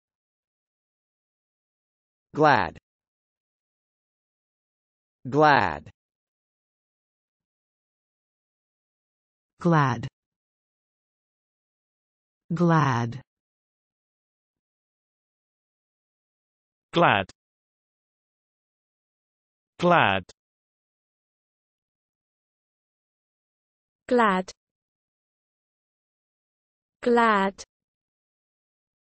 How to pronounce glad
Glad definitions:
    (s): cheerful and bright; "a beaming smile"; "a glad May morning"
    (s): feeling happy appreciation; "glad of the fire''s warmth"
    (a): showing or causing joy and pleasure; especially made happy; "glad you are here"; "glad that they succeeded"; "gave a glad shout"; "a glad smile"; "heard the glad news"; "a glad occasion"
    (s): (`lief'' is archaic) very willing; "was lief to go"; "glad to help"